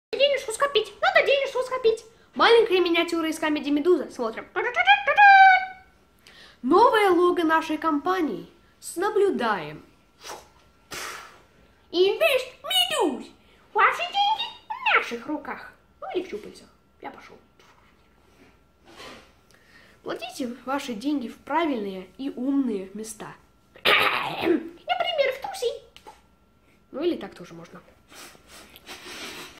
Инвест Медуз Inc.
Новое лого компании Инвест Медуз! Вкладывайте ваши деньги в правильные места!